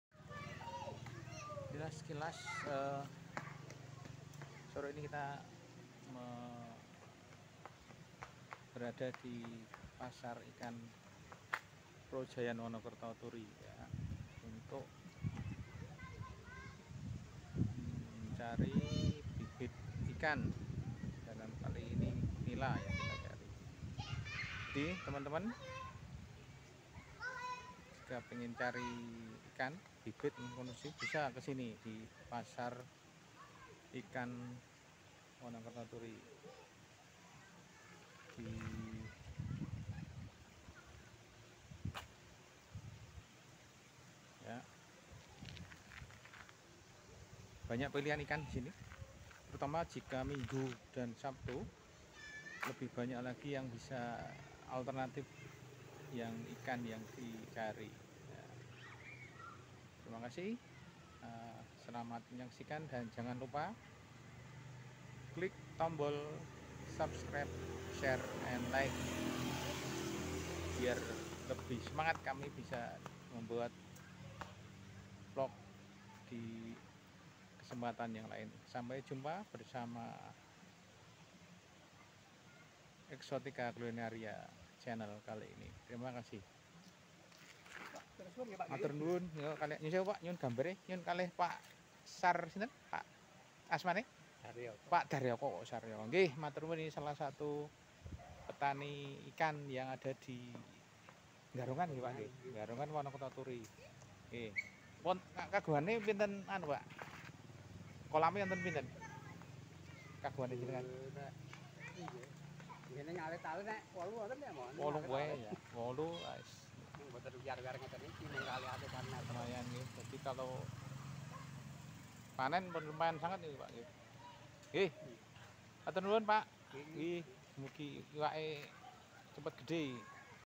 Dolan ke Pasar Ikan Garongan Wonokerto Turi Sleman
Budidaya dan Pasar Ikan Mina Taruna

Di tempat ini terdapat usaha masyarakat Garongan yang bergerak di bidang Perikanan, di sini kita bisa belajar dan melihat secara langsung proses budidaya berbagai macam ikan air tawar secara langsung sampai dengan ke tahapan penjualan hasil perikanan di komplek pasar ikan Garongan. Melimpahnya hasil perikanan menjadikan Desa Wisata Garongan Menjadi daerah penghasil ikan Air Tawar terbesar di kecamatan Turi.